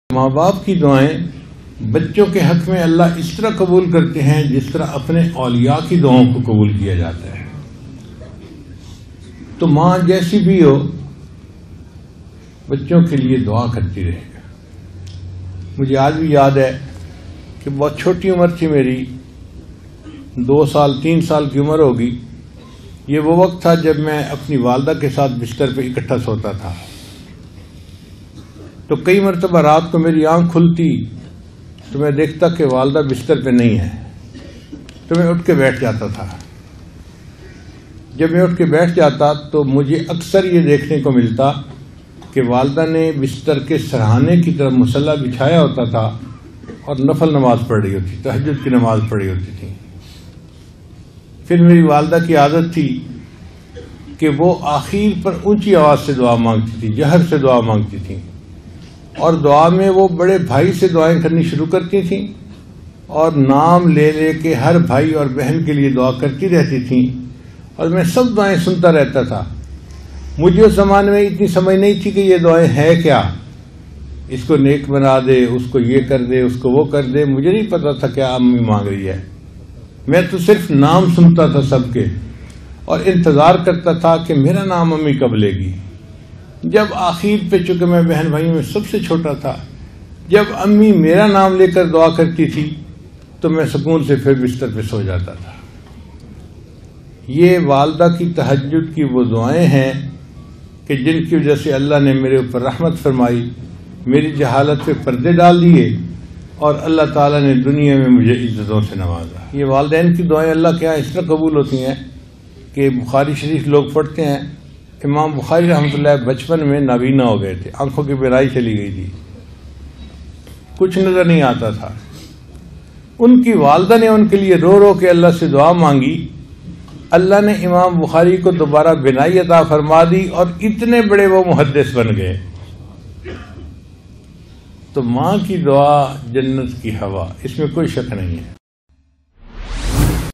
Sacha Waqia - Maa ki Dua ky asrat by Peer Zulifqar Naqshbandi - Imam Bukhari ki Walda ki Dua
کیا آپ جانتے ہیں کہ بچپن میں امام بخاریؒ کی آنکھوں کی بینائی ختم ہوگئی تھی اور وہ کچھ دیکھ نہیں سکتے تھے اور پھر اللہ نے ان کی دعاؤں کی وجہ سے انہیں دوبارہ بینائی عطاء کی ! جانیے پیر ذوالفقار نقشبندی کی زبانی امام بخاریؒ اور اپنی والدہ کی دعاؤں کے واقعات